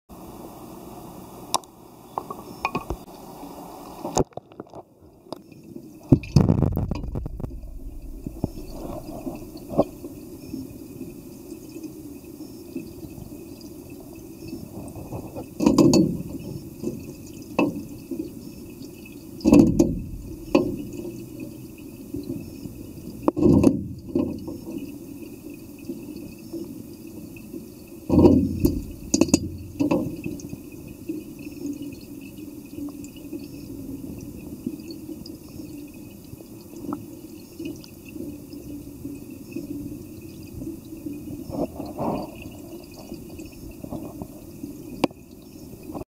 イシガキダイのサルボウ貝捕食シーン 【水中映像】
春の底物釣り定番エサのサルボウガイを食べるイシガキダイ。